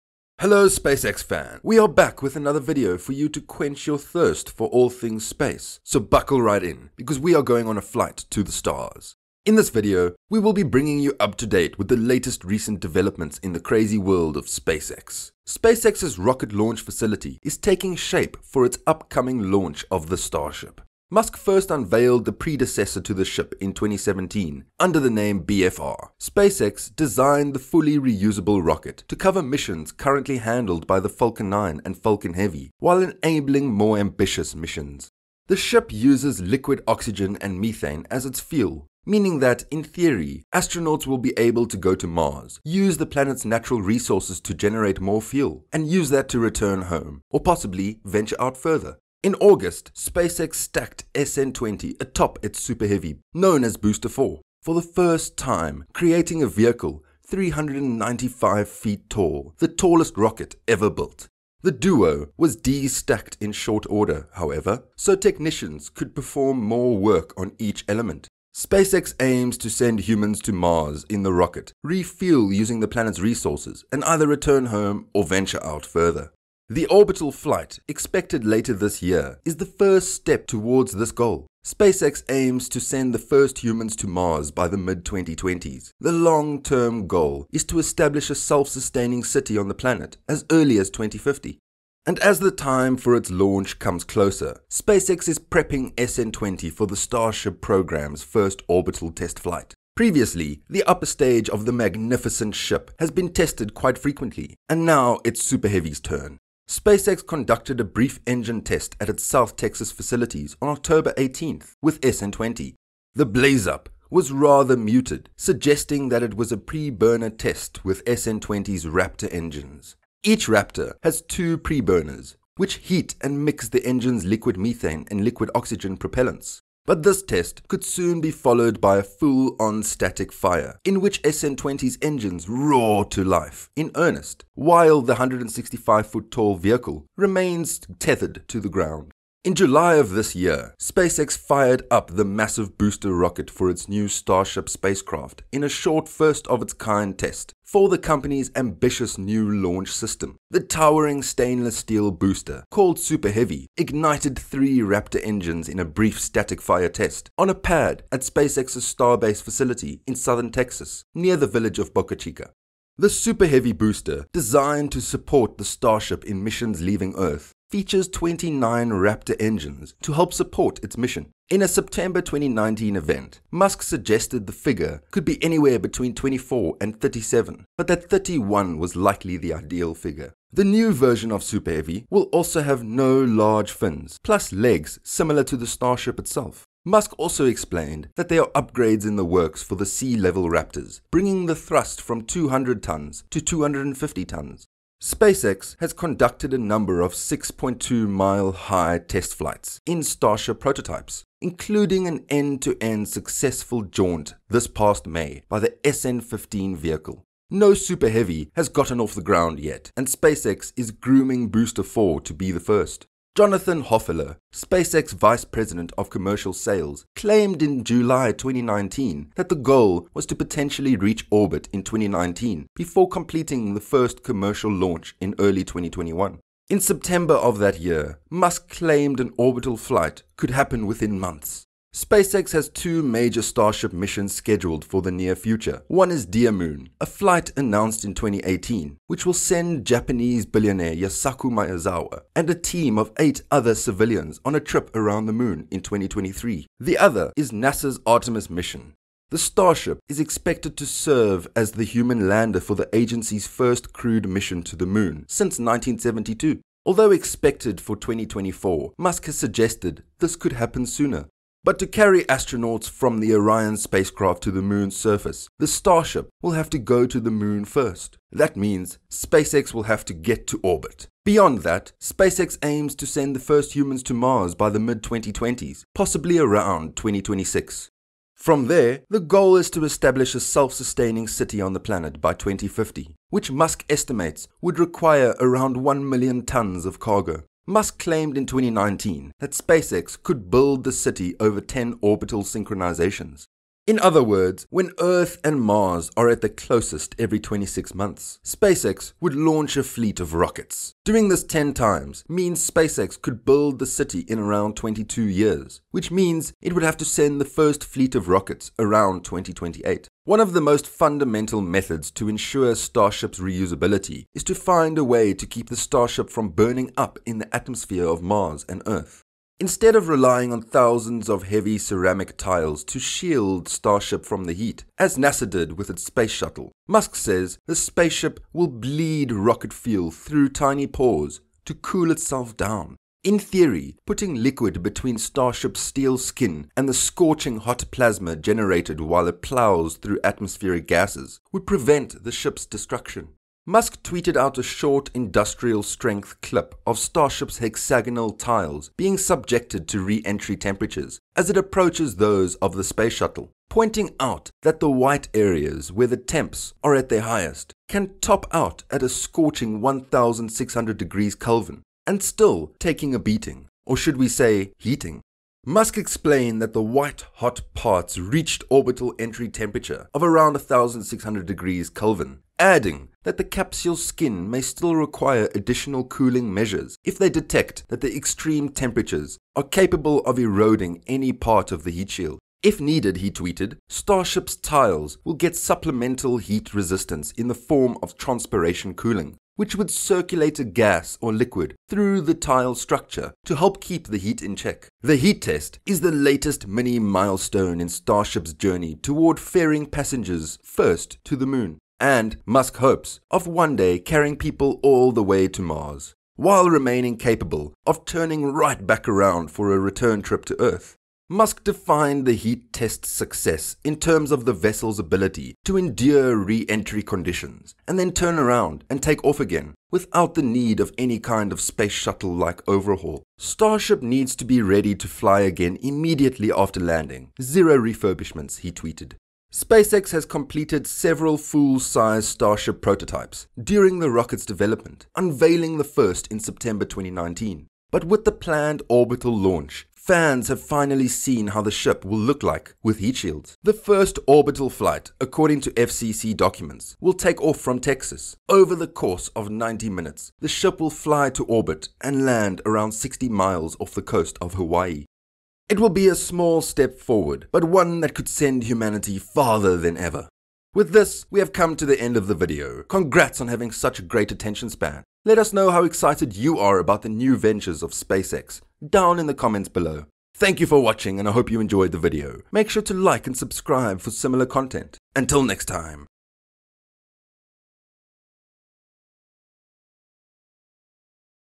Finally! SpaceX NOW Testing Their NEW Super Heavy Rocket
Musk first unveiled the predecessor to the ship in 2017 under the name “BFR”. SpaceX designed the fully-reusable rocket to cover missions currently handled by the Falcon 9 and Falcon Heavy while enabling more ambitious missions. It is designed to take on SpaceX’s existing crew and cargo launches while enabling more ambitious missions.

The ship uses liquid oxygen and methane as its fuel — meaning that, in theory, astronauts will be able to go to Mars, use the planet’s natural resources to generate more fuel, and use that to return home — or possibly venture out further.

In August, SpaceX stacked SN20 atop its Super Heavy, known as Booster 4, for the first time, creating a vehicle 395 feet (120 m) tall — the tallest rocket ever built. The duo was de-stacked in short order, however, so technicians could perform more work on each element.

SpaceX aims to send humans to Mars in the rocket, refuel using the planet’s resources, and either return home or venture out further.

The orbital flight, expected later this year, is the first step toward this goal. SpaceX aims to send the first humans to Mars by the mid-2020s. The long-term goal is to establish a self-sustaining city on the planet as early as 2050.

And as the time for its launch comes closer SpaceX is prepping SN20 for the Starship program's first orbital test flight. Previously the upper stage of the magnificent ship has been tested quite frequently, and now it's Super Heavy’s turn.

SpaceX conducted a brief engine test at its South Texas facilities on Oct. 18 with SN20

The blaze-up was rather muted, suggesting that it was a preburner test with SN20's Raptor engines, as NASASpaceflight's Chris Bergin noted. Each Raptor has two preburners, which heat and mix the engine's liquid methane and liquid oxygen propellants. But this test could soon be followed by a full-on "static fire," in which SN20's engines roar to life in earnest while the 165-foot-tall (50 meters) vehicle remains tethered to the ground.

In July of this year, SpaceX fired up the massive booster rocket for its new Starship spacecraft in a short first-of-its-kind test for the company's ambitious new launch system. The towering stainless-steel booster, called Super Heavy, ignited three Raptor engines in a brief "static fire" test on a pad at SpaceX's Starbase facility in southern Texas, near the village of Boca Chica.

The Super Heavy booster, designed to support the Starship in missions leaving Earth, features 29 Raptor engines to help support its mission. In a September 2019 event, Musk suggested the figure could be anywhere between 24 and 37 but that 31 was the likely ideal figure. The new version of the Super Heavy will also have no large fins, plus legs similar to the Starship itself. Musk also explained that there are upgrades in the works for the sea-level Raptors, bringing the thrust from 200 ton to 250 ton.

SpaceX has conducted a number of 6.2-mile-high (10 kilometers) test flights with Starship prototypes, including an end-to-end successful jaunt this past May by the SN15 vehicle. No Super Heavy has gotten off the ground yet, and SpaceX is grooming Booster 4 to be the first.

Jonathan Hofeller, SpaceX vice president of commercial sales, claimed in July 2019 that the goal was to potentially reach orbit in 2019 before completing the first commercial launch in early 2021. In September of that year, Musk claimed an orbital flight could happen within months.

SpaceX has two major Starship missions scheduled for the near future. One is the “dearMoon” flight announced in 2018, which will send Japanese billionaire Yusaku Maezawa and a team of eight other civilians on a trip around the Moon in 2023